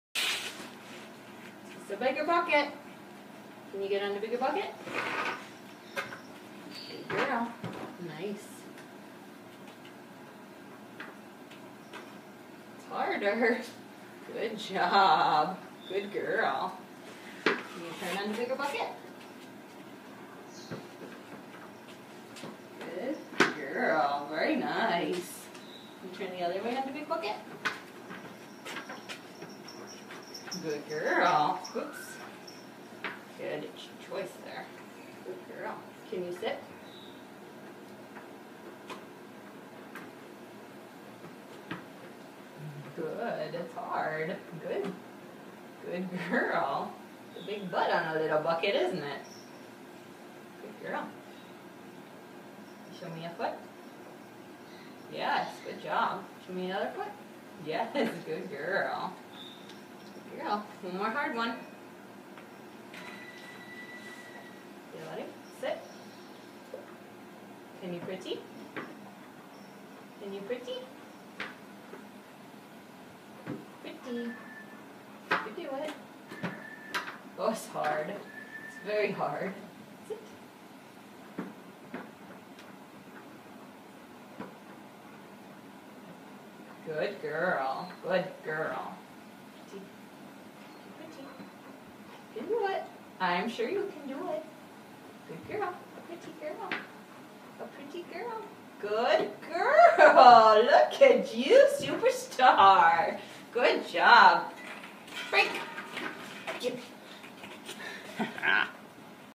P's Bucket Trick
Started with a short bucket and graduated to the trickier, less stable tall bucket.